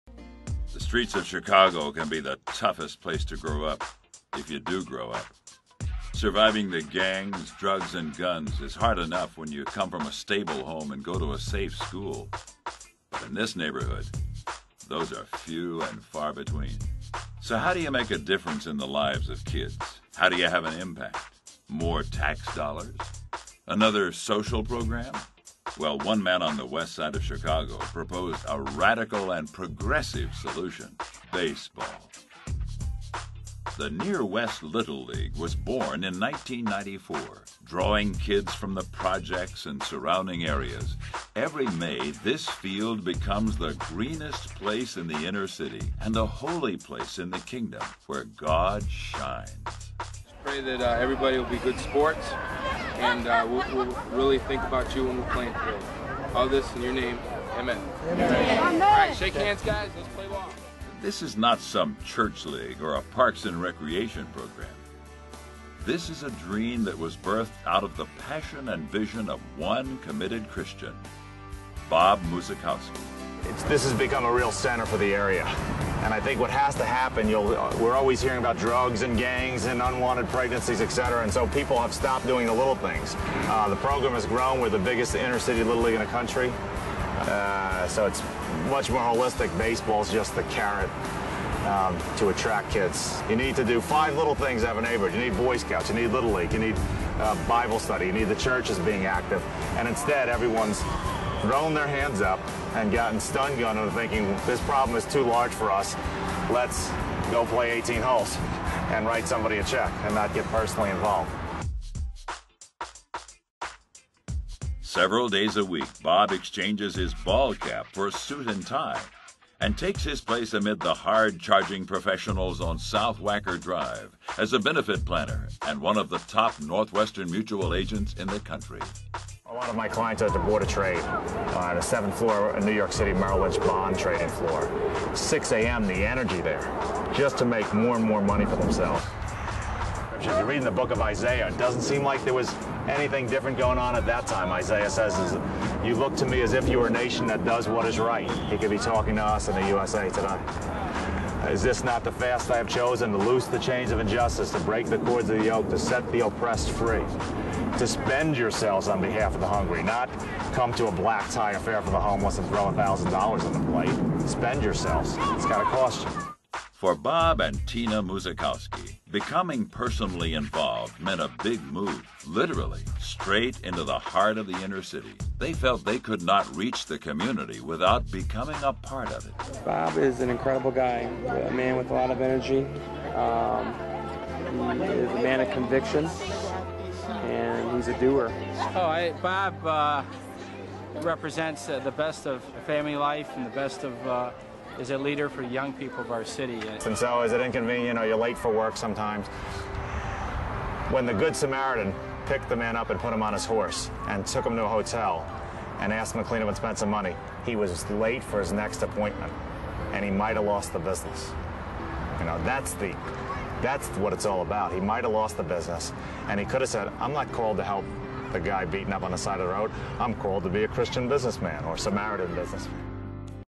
Bob Muzikowski
Bob shares how Halftime impacted his life